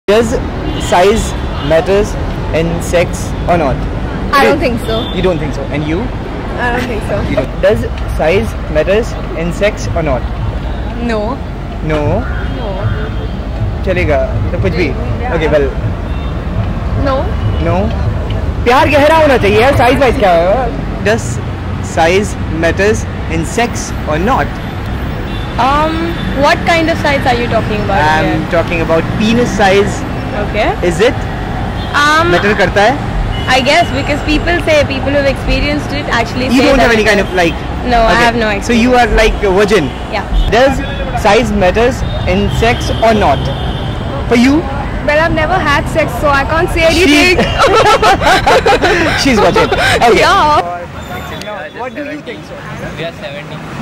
modern and open minded girls reaction about penis size...
mumbai girls reaction about this question is penis size matter during sex or not... ???